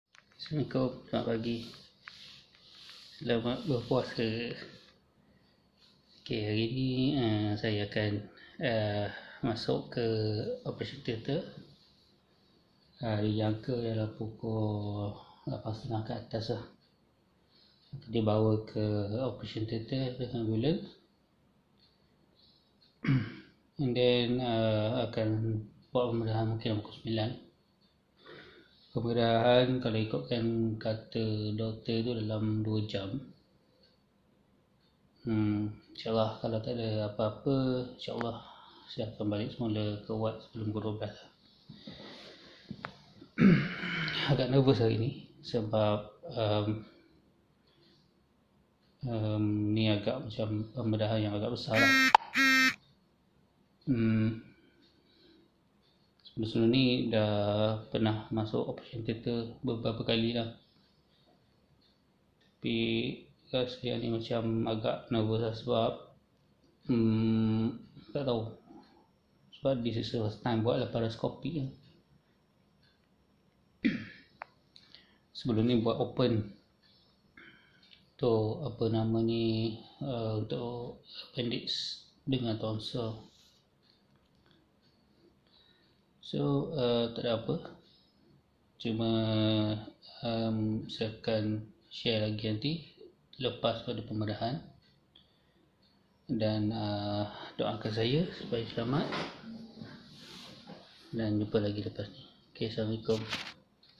Laparoscopy Nissen Fundoplication - OT Pre-entry
Hari ini 16/4/2021, saya akan membuat pembedahan NF. Di jangka akan masuk ke OT pkl 9 pagi.

laparoscopic nissen fundoplication. magnetic sphincter augmentation
laparoscopic nissen fundoplication. magnetic sphincter augmentation (linx)
open vs. laparoscopic nissen fundoplication